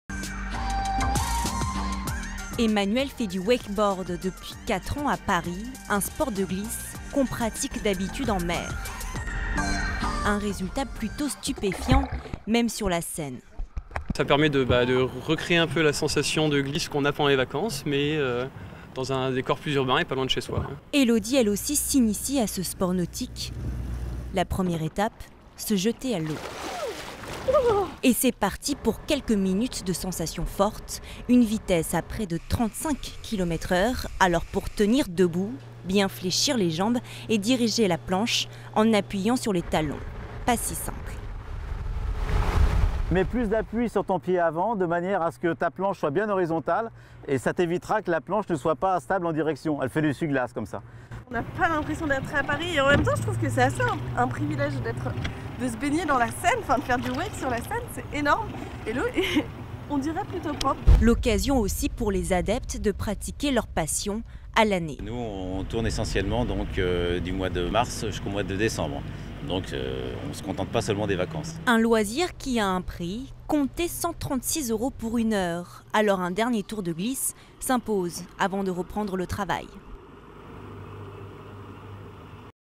Du wakeboard ... sur la Seine
De plus en plus de Français sont devenus adeptes du wakeboard, un sport nautique le plus souvent pratiqué en mer. Mais pour ceux qui ne partiront pas en vacances cet été, un club propose de se faire tracter ... sur la Seine.